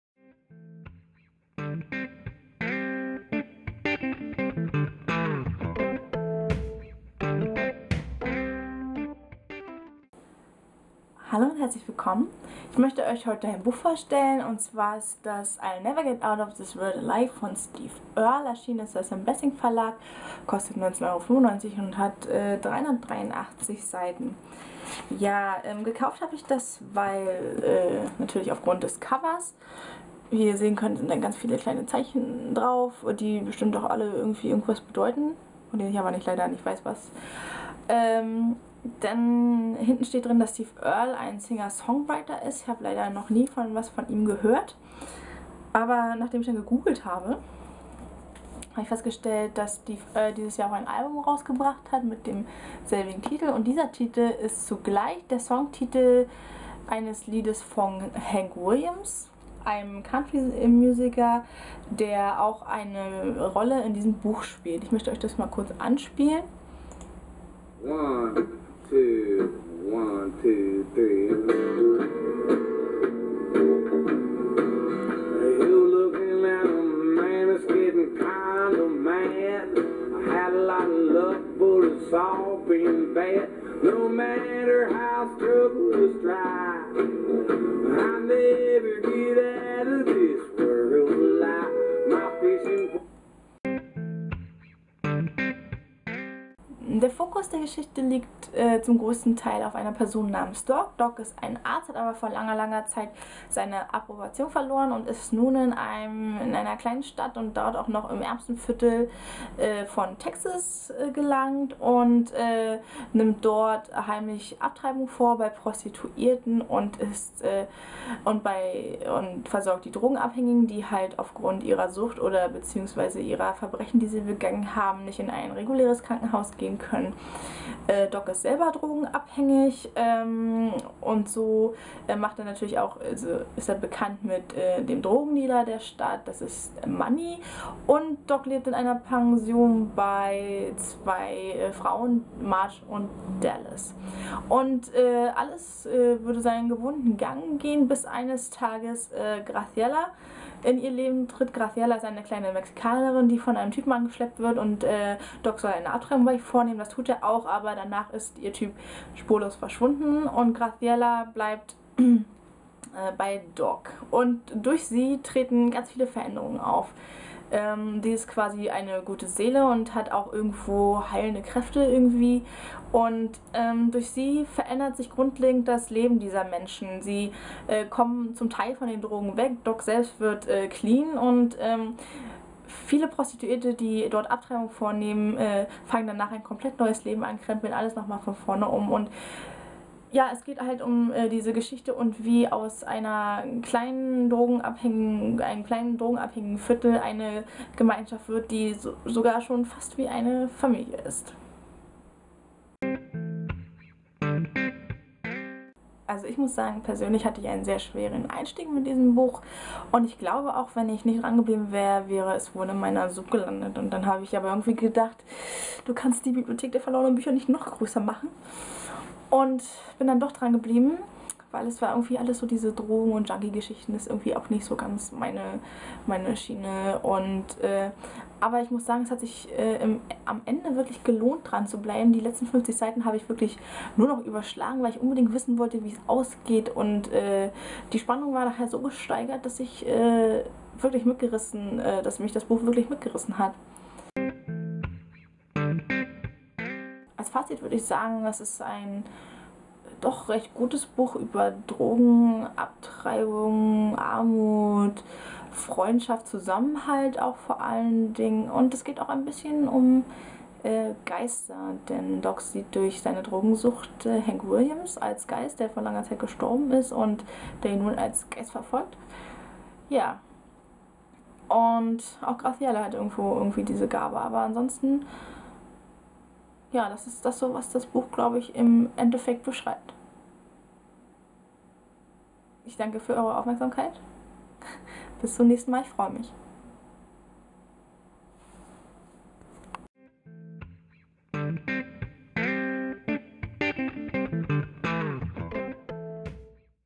I'll never get out of this world alive - Steve Earle
Ich glaube das ist der Debütroman von Steave Earle, ein bisschen Western, ein bisschen Underground, ein bisschen Country, eine bisschen Übernatürliches, ein bisschen von allem in einem Buch.